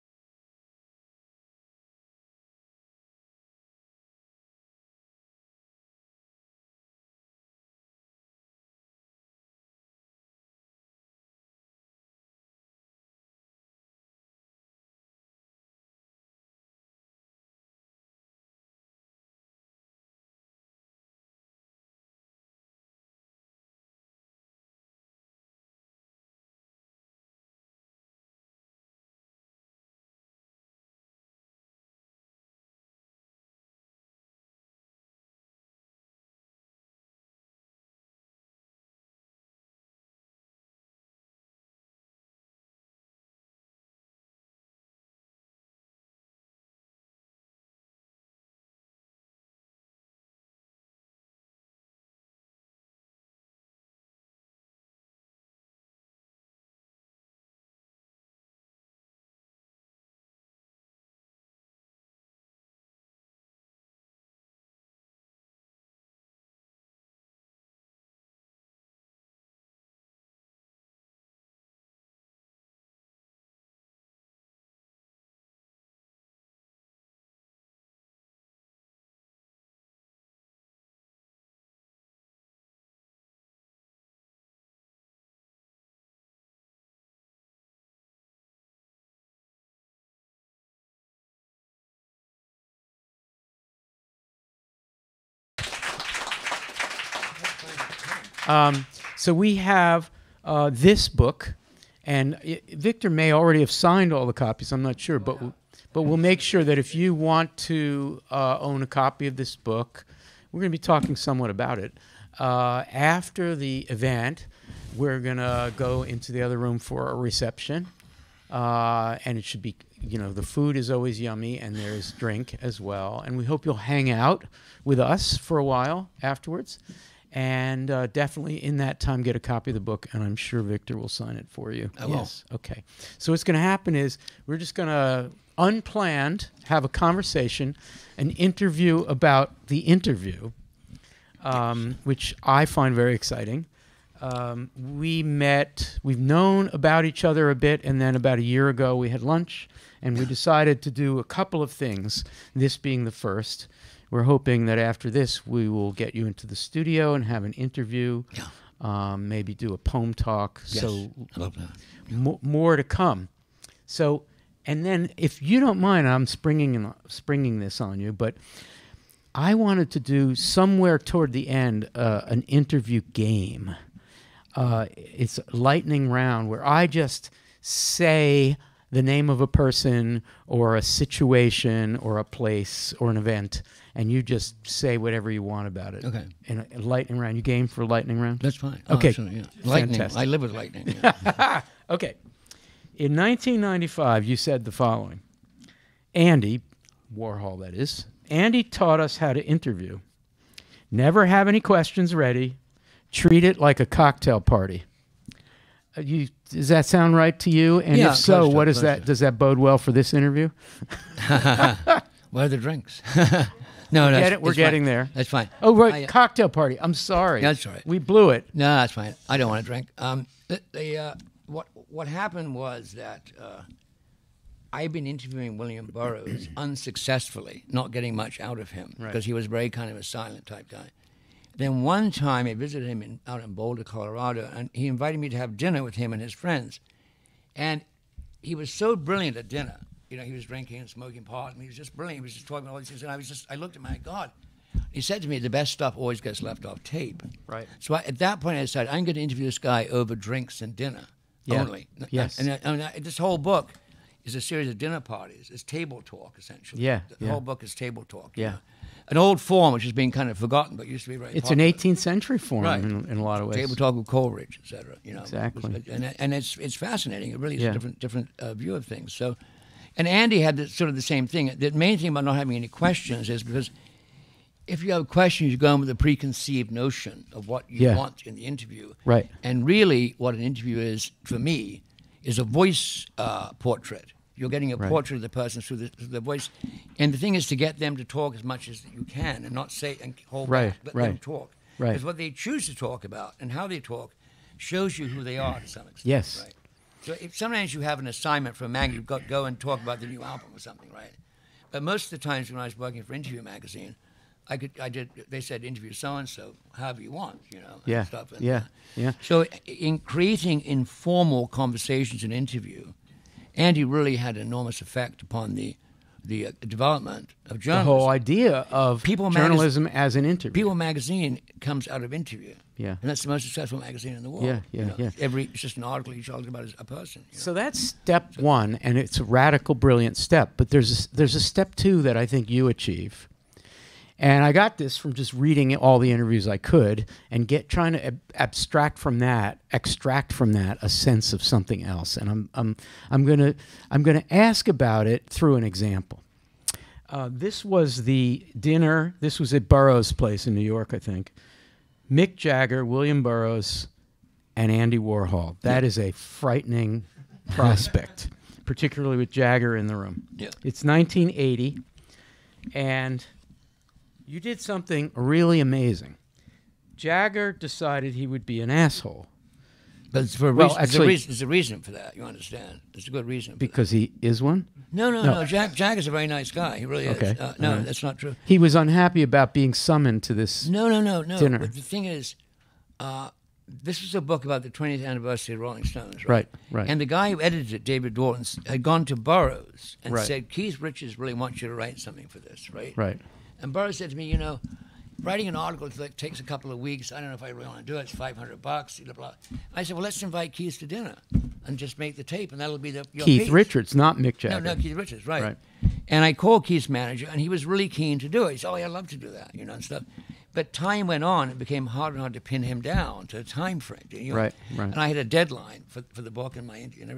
An interview about the interview (part 1 of 2)
Victor Bockris in conversation with Al Filreis

Time-stamps:
0:00 Pre-event
2:13 Program

Victor Bockris graduated from the University of Pennsylvania in 1971. After graduation, he founded Telegraph Press, a seminal small press in the early seventies. He also had several books of poems and prose published, including In America and The Joe DiMaggio Victor Bockris Special. He worked with collaborator Andrew Wylie under the name “Brockis-Wylie” to publish a number of interviews under the column “Electric Generation” for The Drummer. Their crowning achievement was Ali: Fighter Poet Prophet, published by the legendary Maurice Girodias on the day Ali regained his heavyweight crown in October 1974. After the duo broke up amicably, Bockris went on to publish widely in Interview and High Times. He worked freelance for Andy Warhol and William Burroughs and became a fixture on the punk scene.

 In the 1980s he published a trilogy of portraits: A Report from the Bunker With William Burroughs, Making Tracks: The Rise of Blondie, and Uptight: the Velvet Underground Story. In the 1990s, he turned to biography, publishing the trilogy Warhol: The Biography, Keith Richards: The Biography, and Transformer: The Lou Reed Story.

Bockris’s twenty-seven year run as the poet laureate of the New York Underground led him to develop the book Beat Punks: New York's Underground Culture from the Beat Generation to the Punk Explosion.